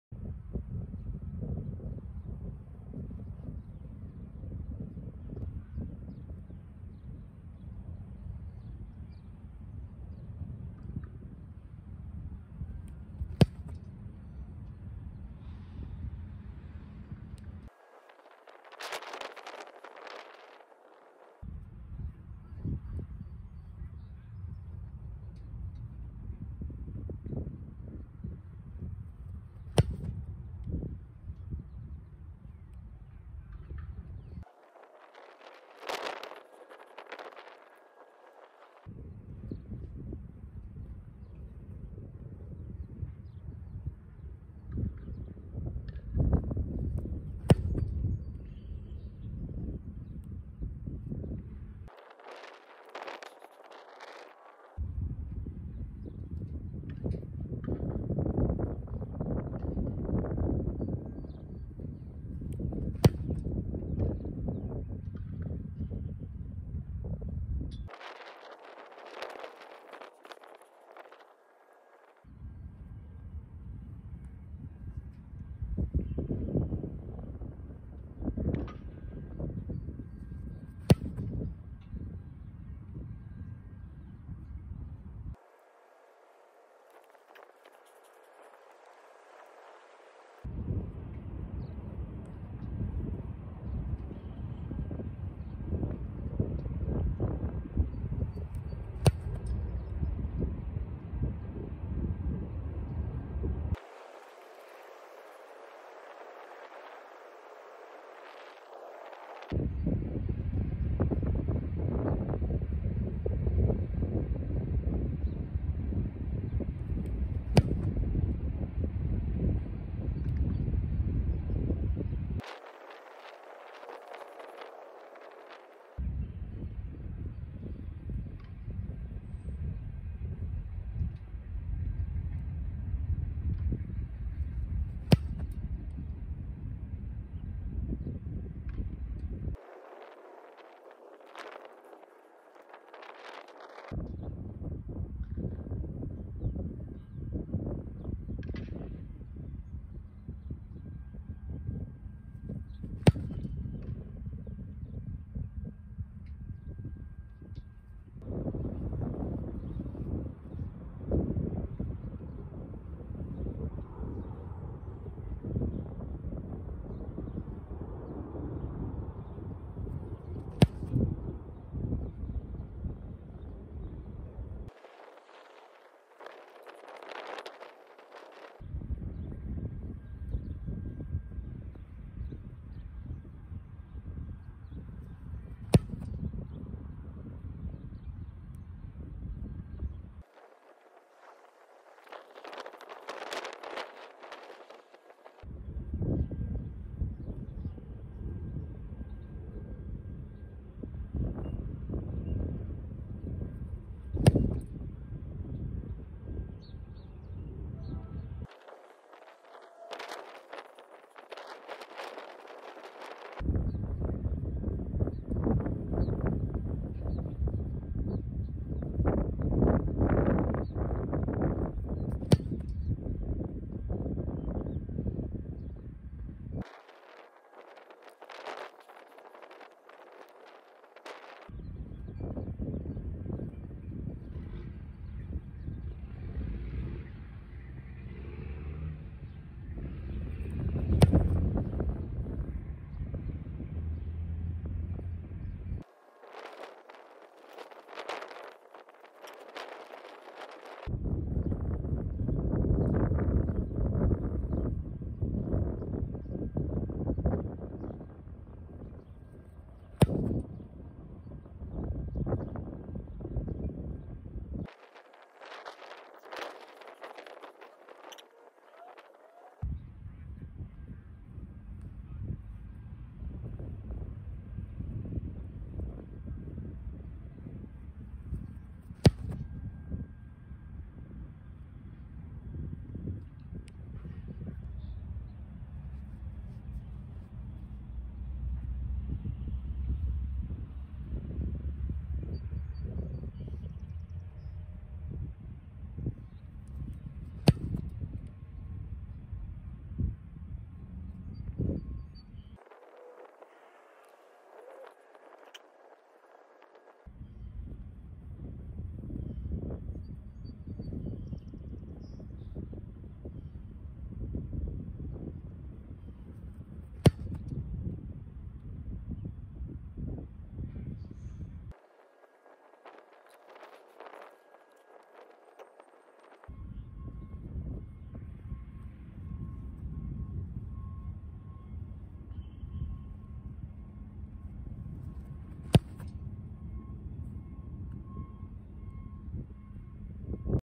Taylor Bertolet - Kicking Training (3/25/22)
Location: Kean University (NFL Uprights)
Date: 3/25/22